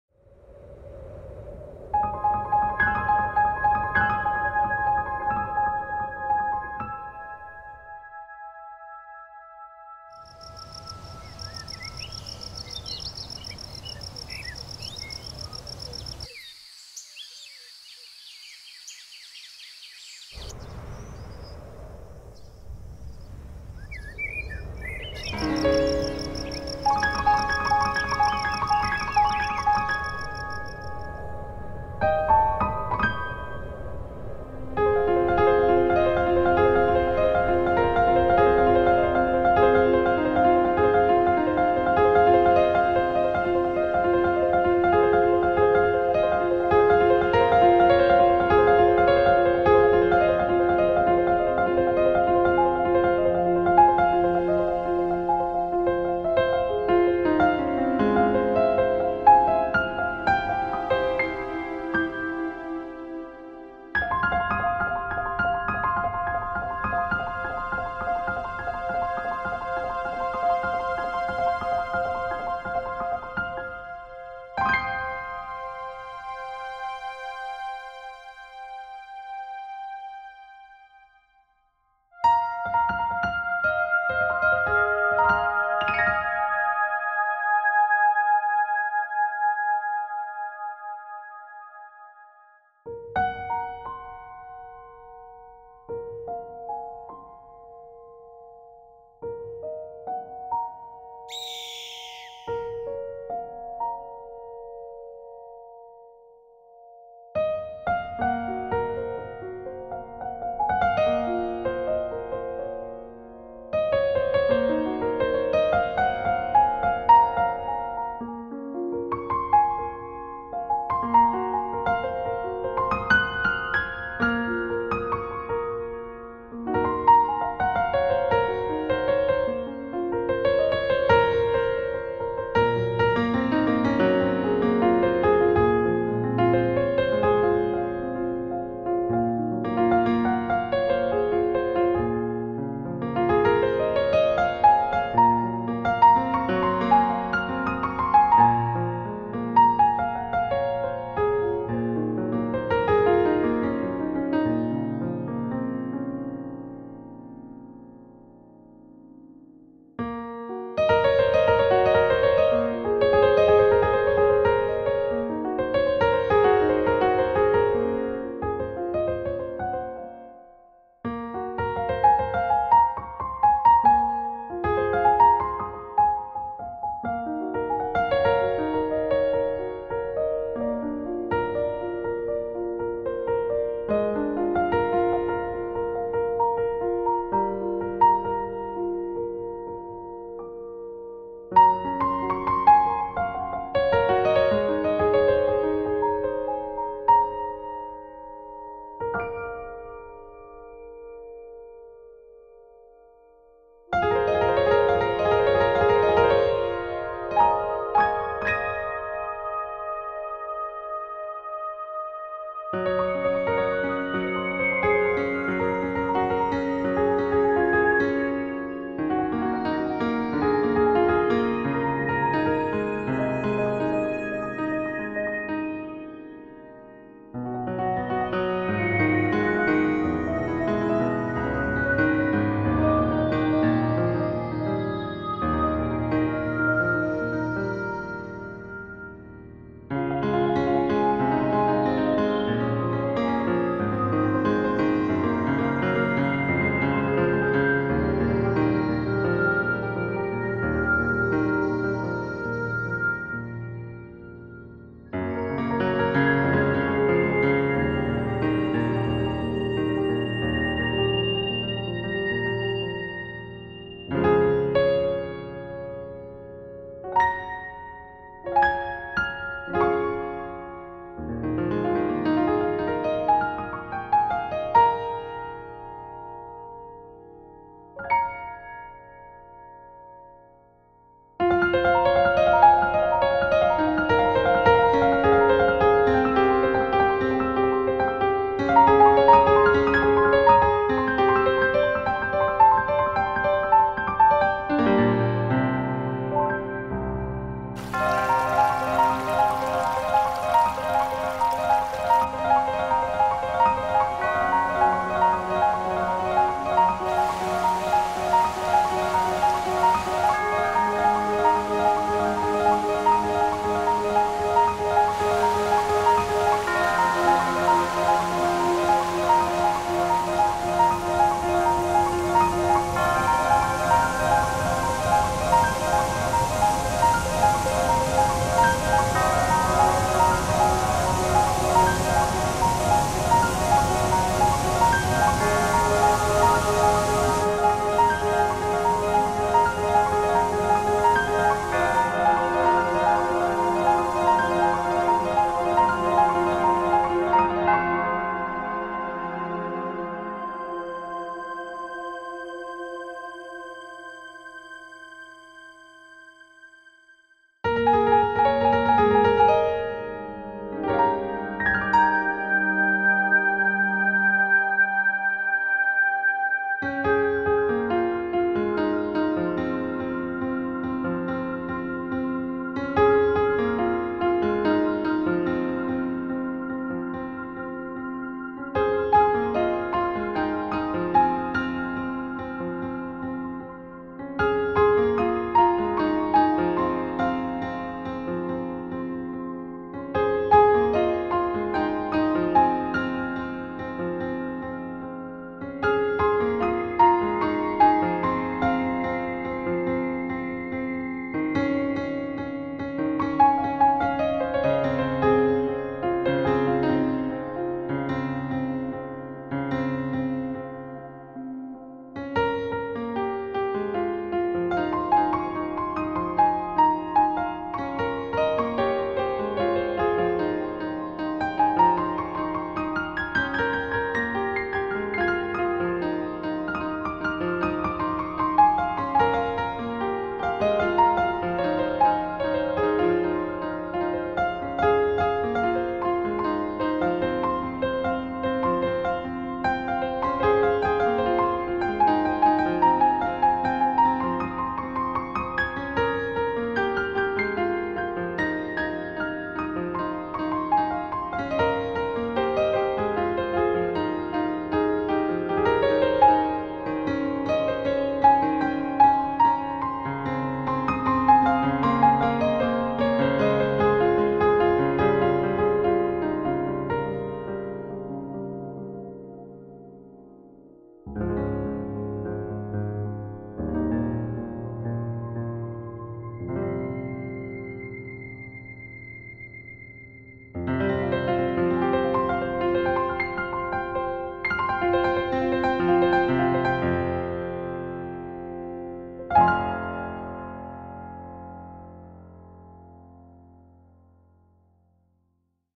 Awakening, Ascending, Returning"
Drone video of Georgia, Florida, and North Carolina.